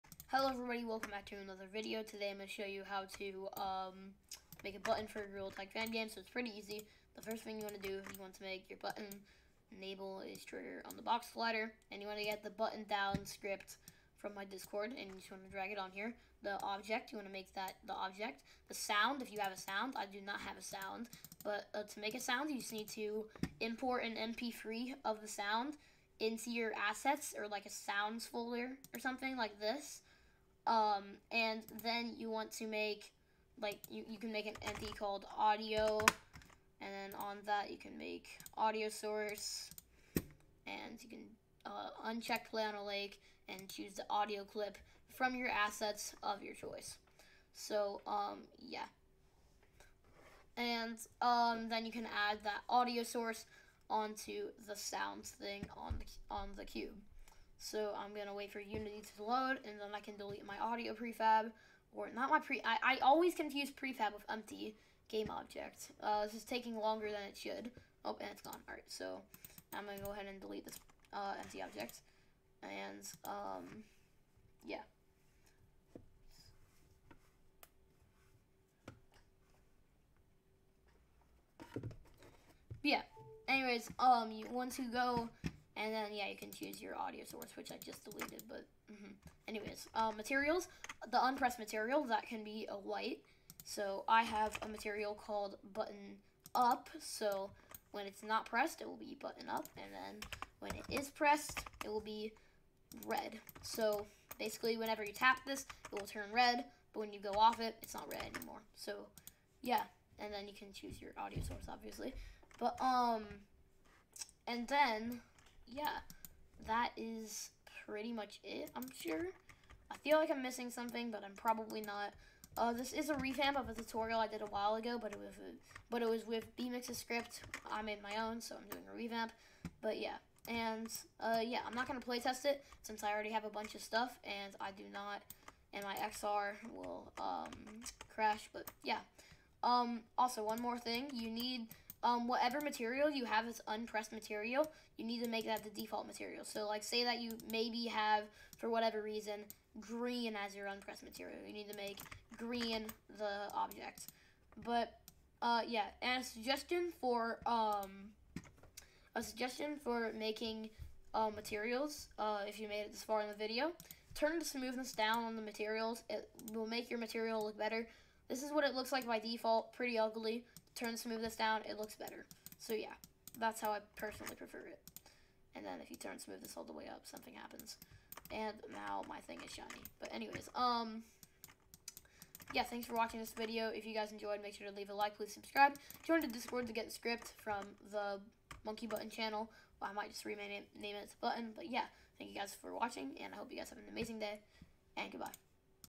How to add a button to your Gorilla Tag Fan Game! (revamped)
comment if discord link is invalid and ill give you a new one :D